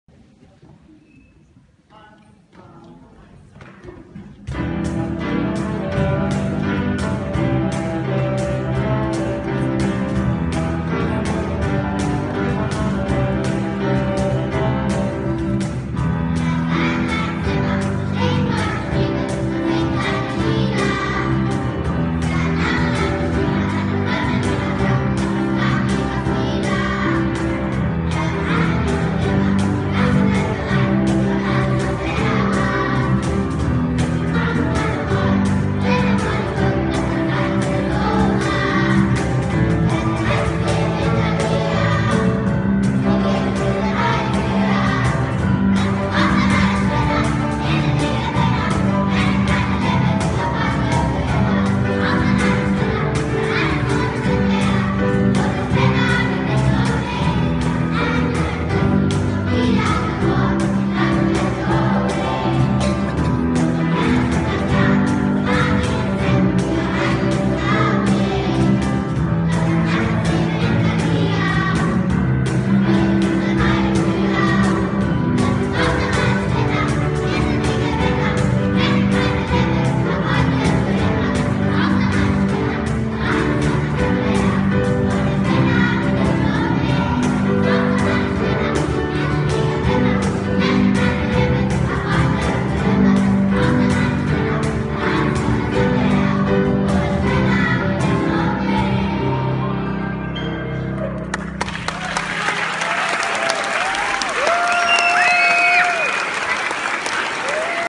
Schule Kerschensteinerstrasse singt am Friedrich-Ebert-Gymnasium